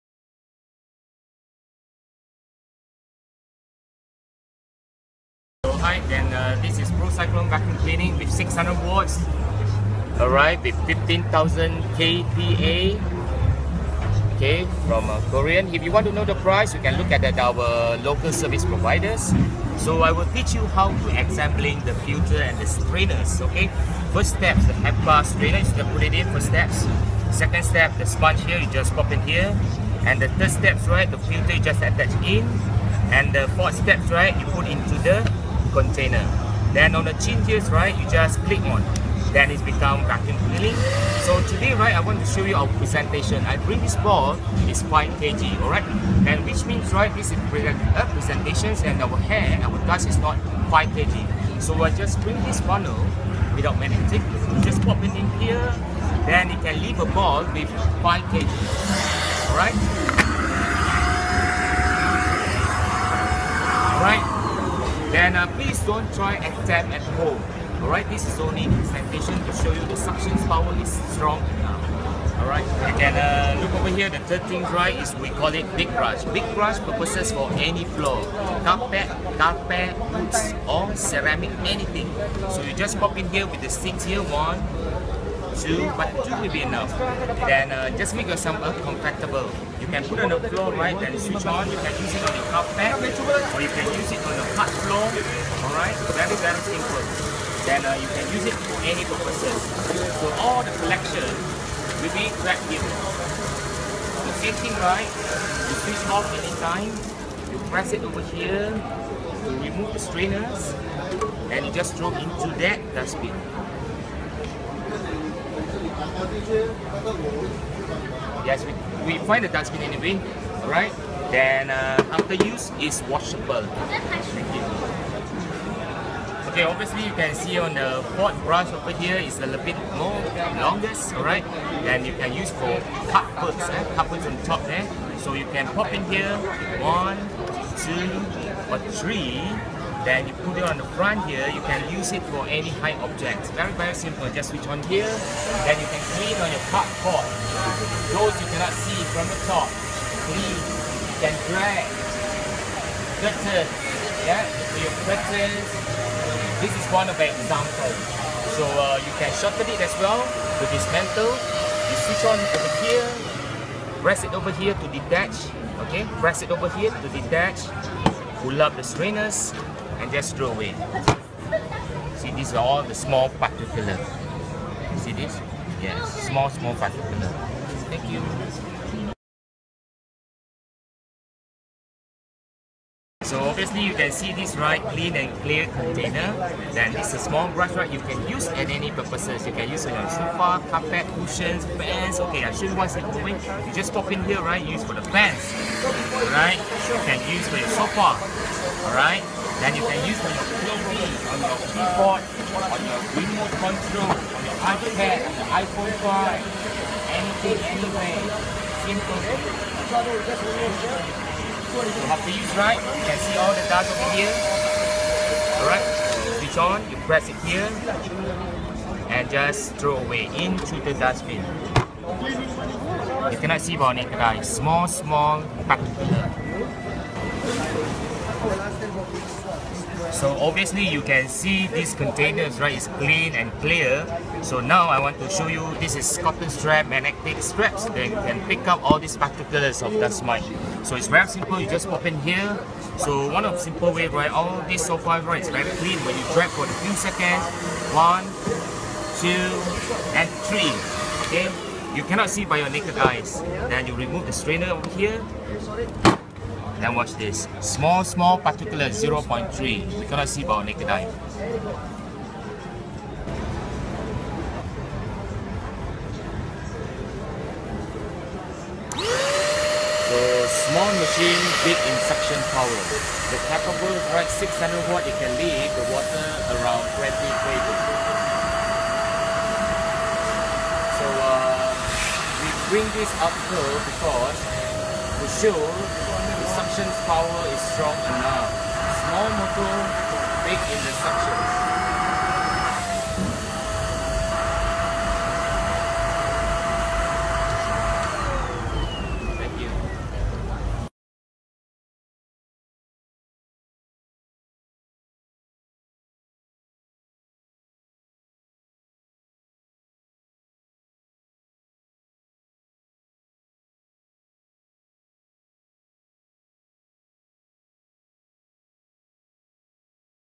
Shimono PRO-Cyclone Rocket Vacuum SVC 1015
现在购买优惠价RM2xx Only !

新型号料 Svc1015 不锈钢(Aluminium)filter ,不需要换纲了。
过滤网是白钢的哦，也有改良一些独特新功能，旋风功能吸力更强了，是白钢过滤网有四层过滤，也更耐用无须更换可以长期使用，可以省下来换过滤网的费用。。而且集尘盖是Polycarbonate品质，掉地下沒那么容易爆裂。吸力15000PA suction power

SVC1015 的特点:
1）新款svc1015 白钢纲就不用时常换，可以省下买纲费用。。 因为白钢纲比较耐用，不需要换了。
有四层过滤。
2）而且dustcontainer 是Polycarbonate品质，掉地下沒那么容易爆裂。
3）Dustcontainer 可以support 80 kg 重量.
4）除非敲到凹凸了，全部零件坏了有得购买的。包括马达都有得替换。

P/s: 优惠数量有限！！先到先得！！！

（这款是国外版SVC1016,功能一样，只是是外形集尘盖有点不同）

Wechat ID : Allychang3355 & Milkmilk3435889